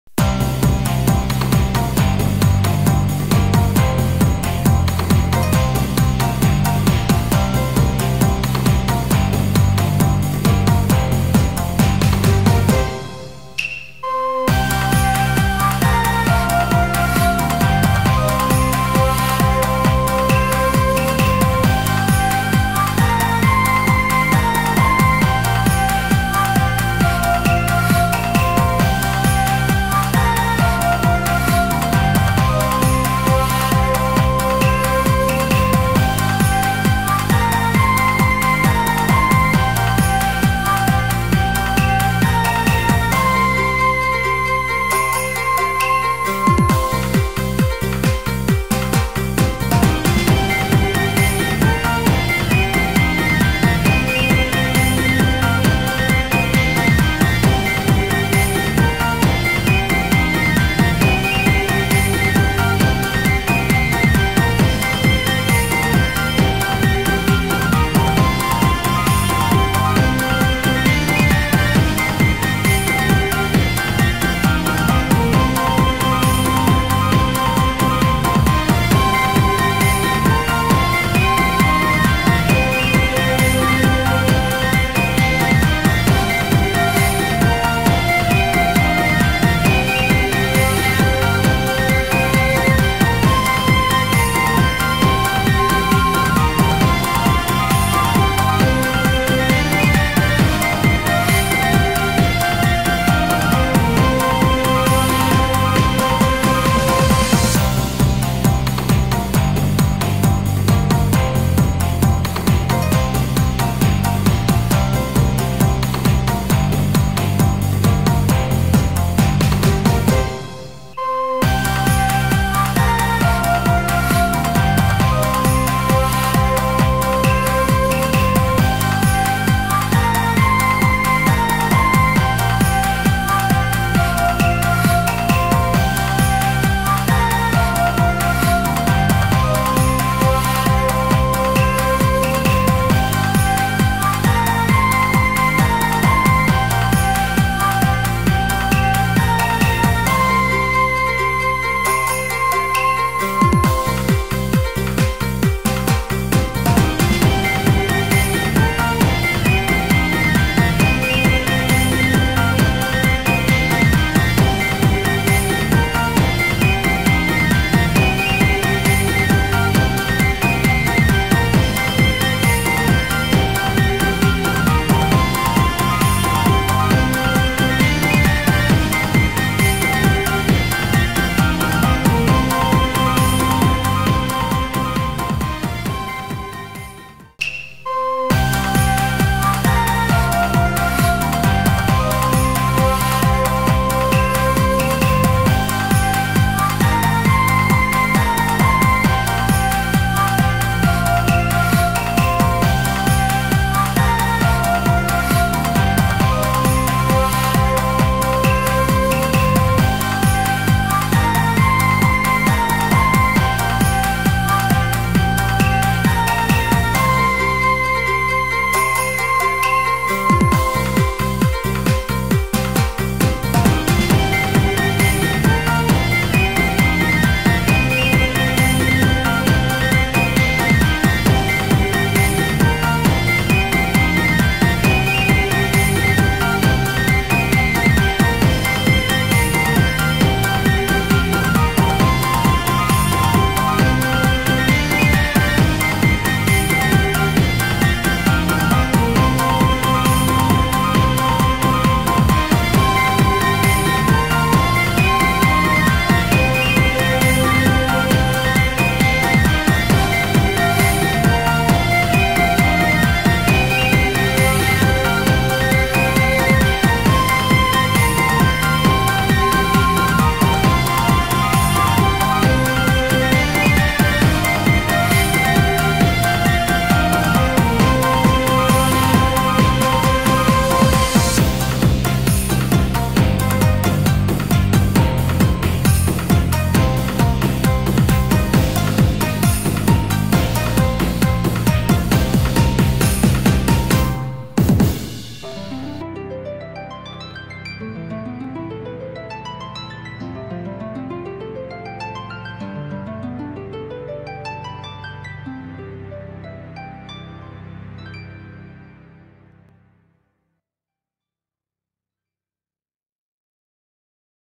Sea Surface Temperature Anomaly : Jul.1, 2016 - Jan.6, 2018
Concern of influence on fish sensitive to environmental change / Su sıcaklığı hassasiyet 0.03℃ olan çevresel değişime duyarlı olan balıkların etkisi endişeliyiz / 環境の変化に敏感な魚に及ぼす影響が心配